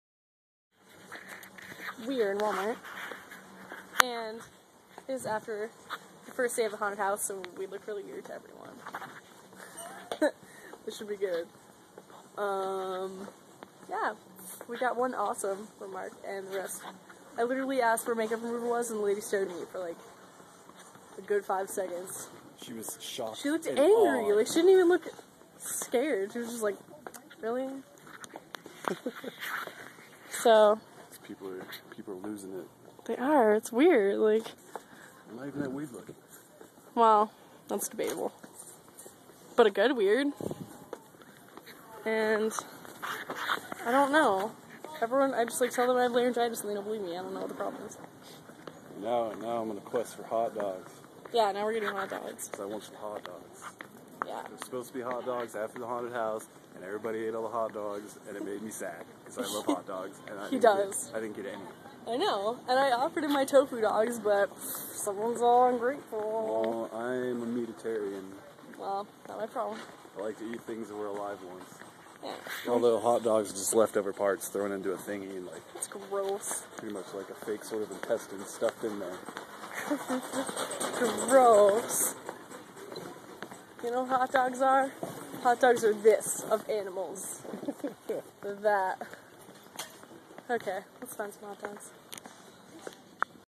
Walmart after haunted house
We had to stop at walmart after working the opening night of Terror in the dark. Apparently we look strange.